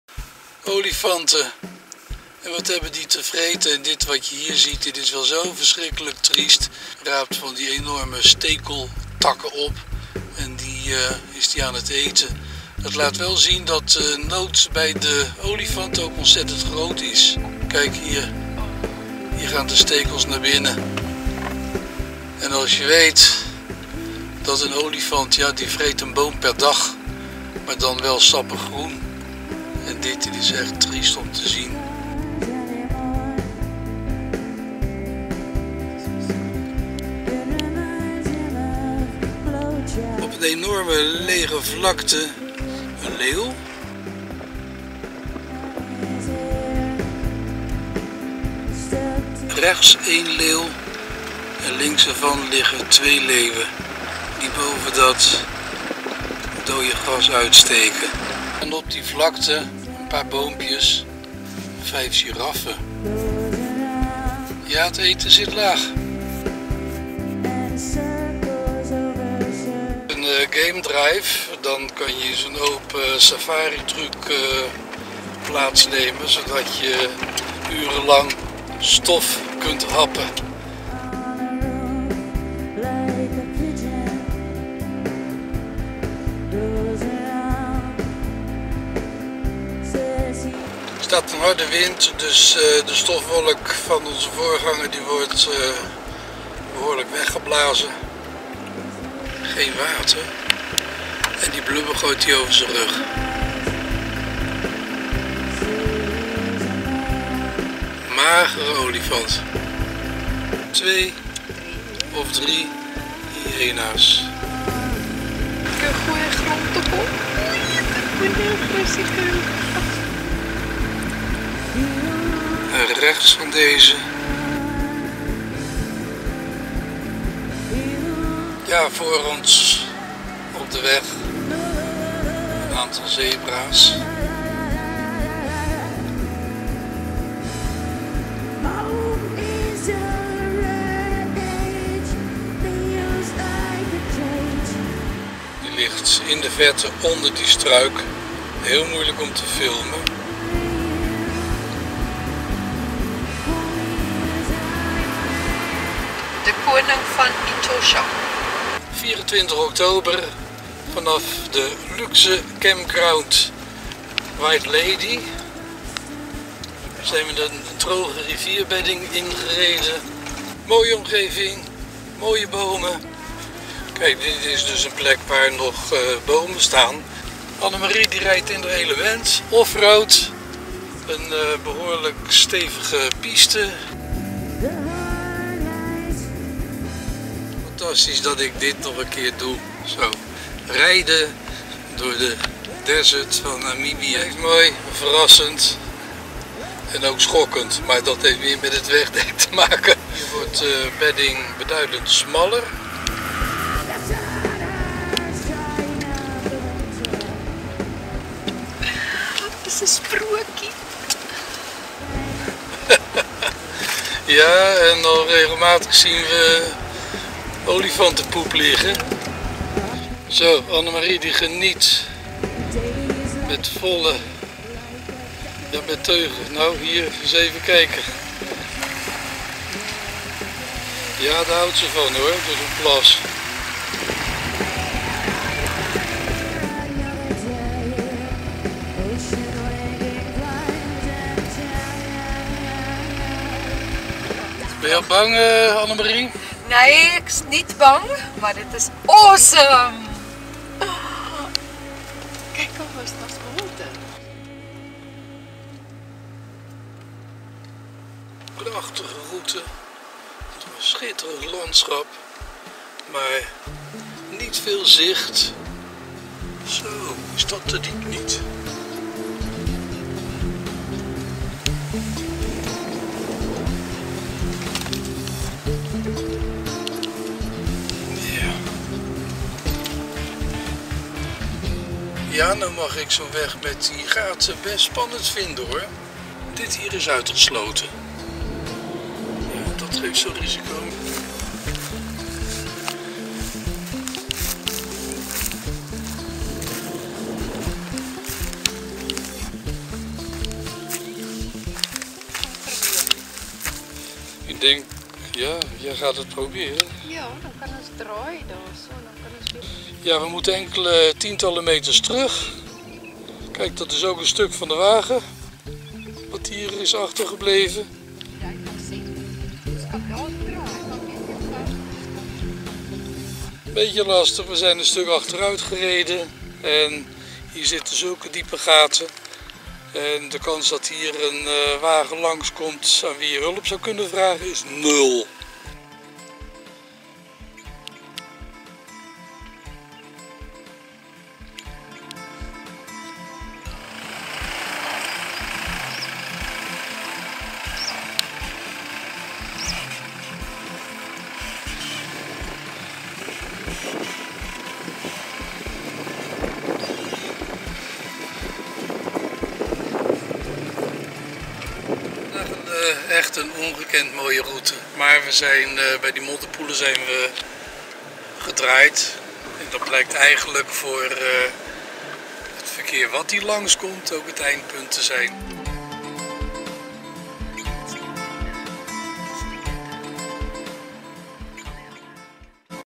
2019. Van Etosha naar White Lady.
In Etosha zien we met verbijstering hoe olifanten dode doorntakken eten. We rijden door de kleurloze vlakte van de Etosha Pan en dan gaan we naar White Lady, waar we offroad door een rivierbedding gaan.